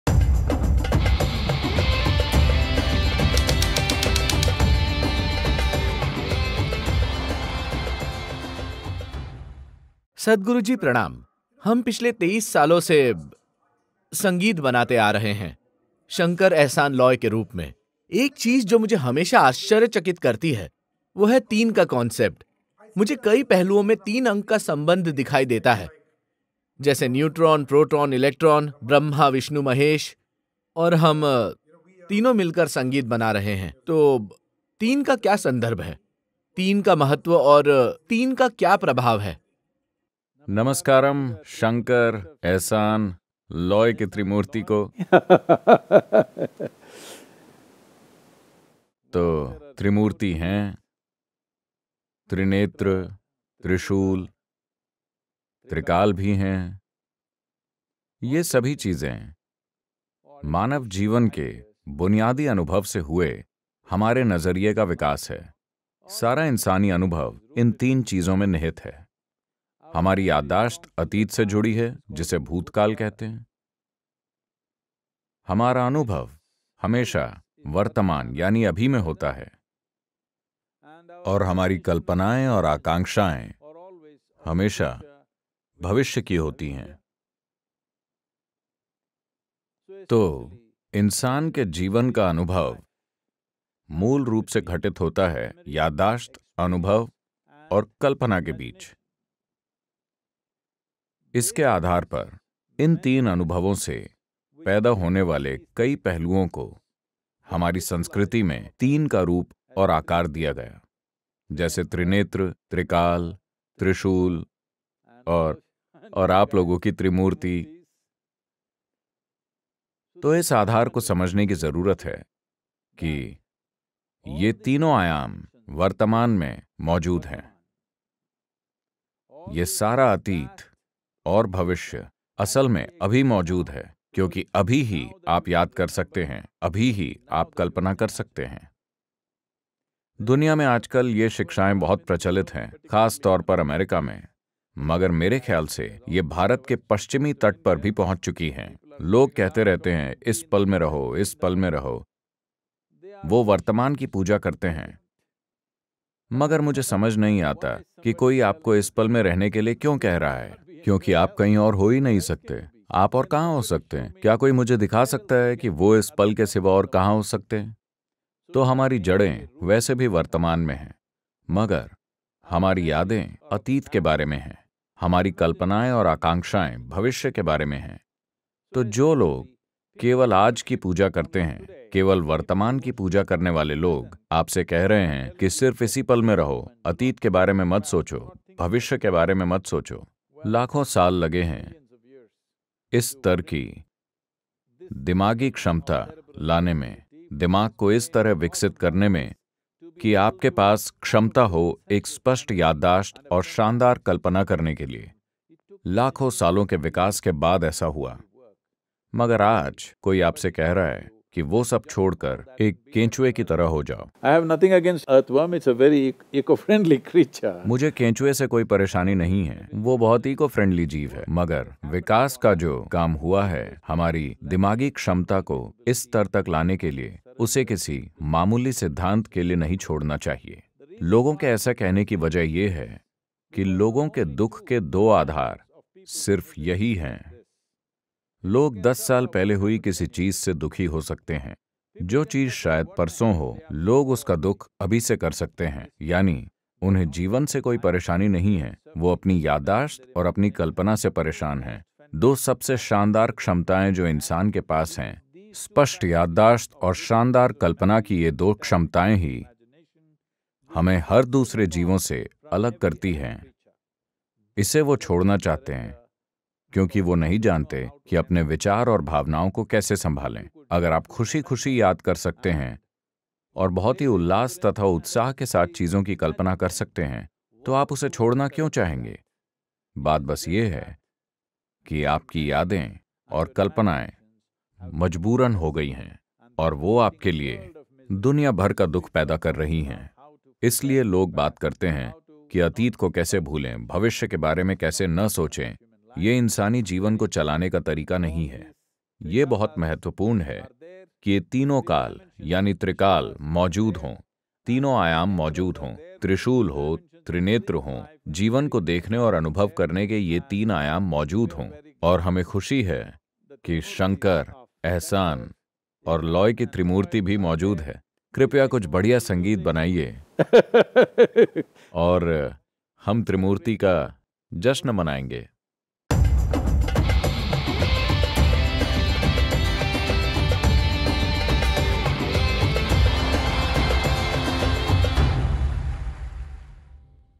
नम्बर 3 का क्या महत्व है? शंकर महादेवन ने पूछा सद्‌गुरु से
इलेक्ट्रान, प्रोटोन और न्यूट्रॉन हो या ब्रह्मा विष्णु और महेश - जीवन में 3 के अंक से कई बार हमारा सामना होता है। नंबर 3 का क्या महत्व है?

सद्‌गुरु से अपना सवाल यहाँ पूछें: UnplugWithSadhguru.org